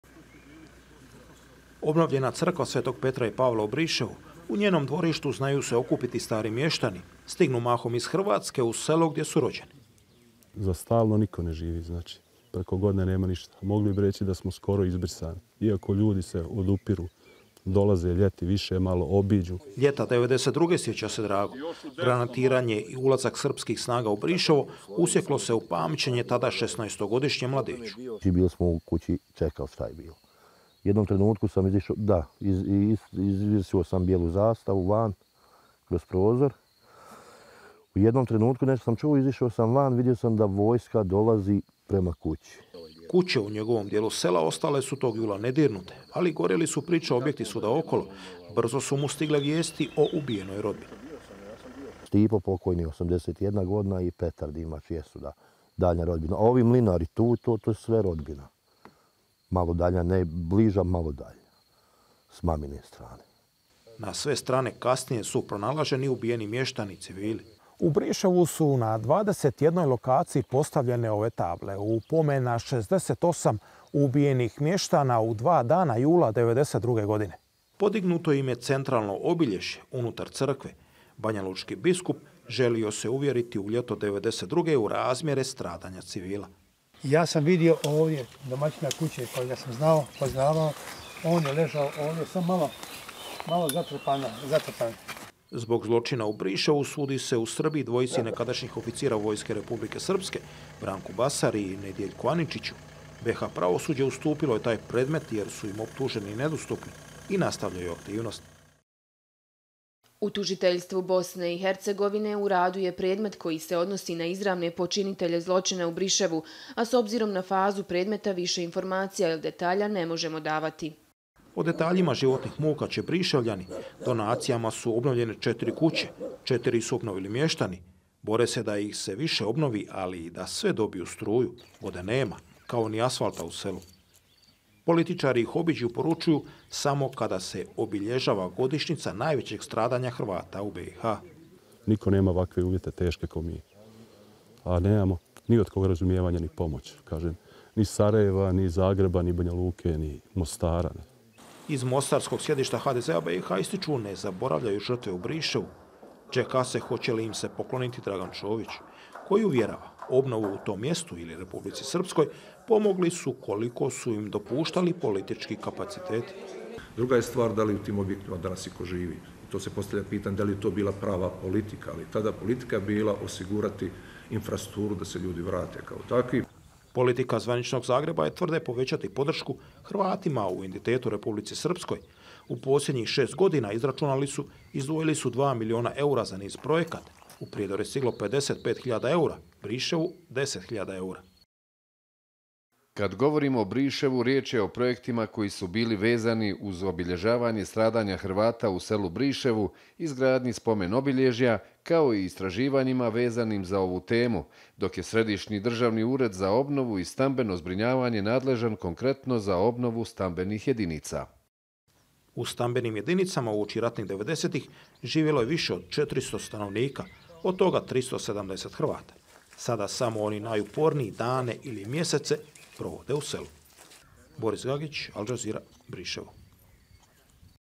Briševljana se sjete samo na godišnjicu velikog zločina
Mjesto bez stalnih mještana, tako danas izgleda Briševo kod Prijedora.

U dva dana jula 1992., 68 civila, Hrvata, ubile su srpske snage.

Čeka se procesuiranje direktno odgovornih.

Rođeni Briševljani nezadovoljni su odnosom visoke politike iz BiH i Hrvatske prema njima.

Sjete ih se, kažu, tek povremeno.